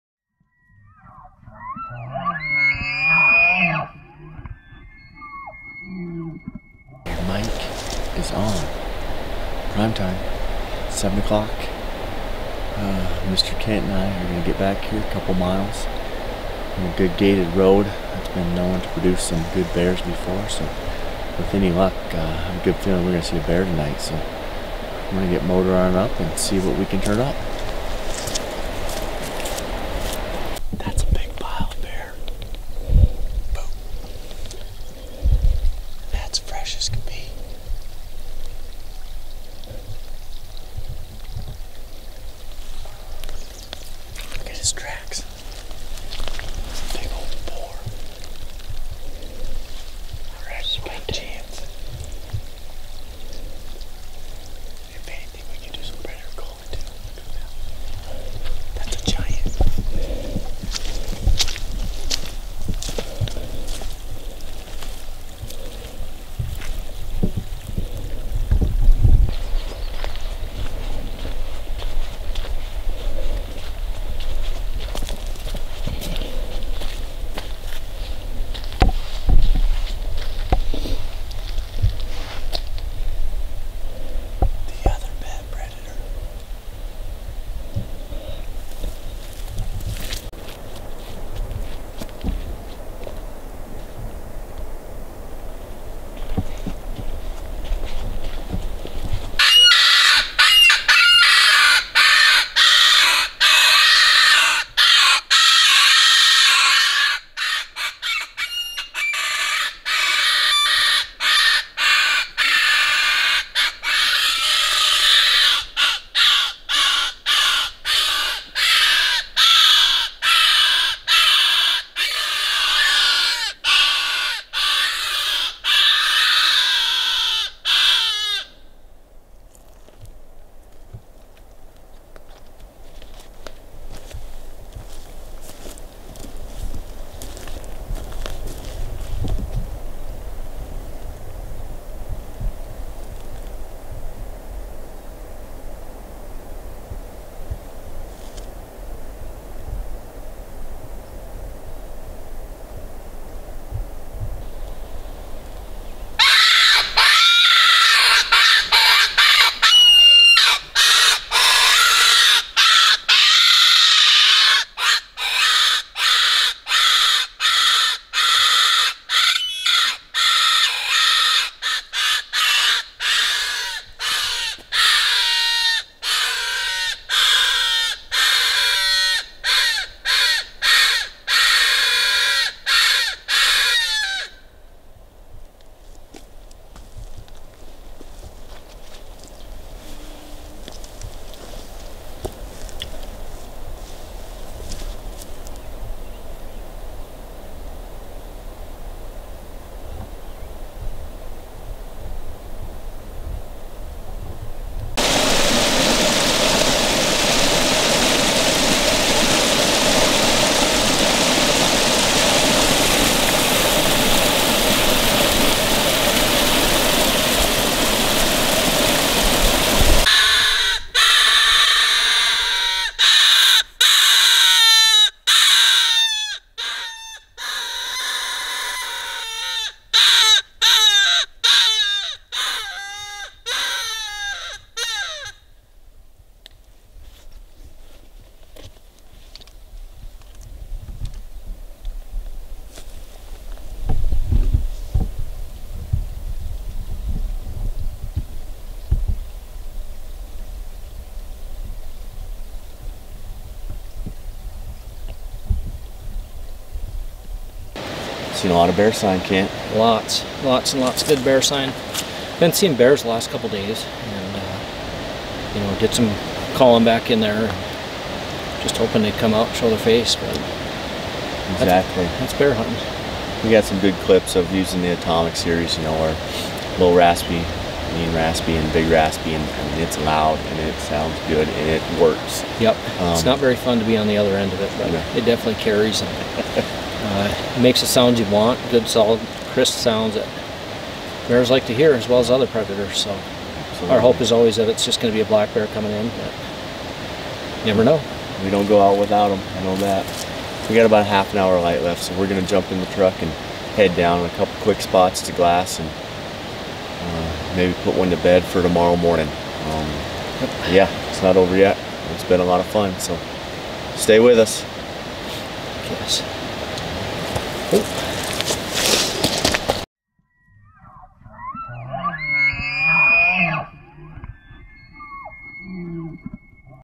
Bear Tips 2022 RMHC
Our own Josh Fields and Kent Anderson spent some time this spring chasing bears in Montana.  Although no bears were killed we are here to show you our Atomic 13 Predator line and effective ways to lure in a bruin to your location!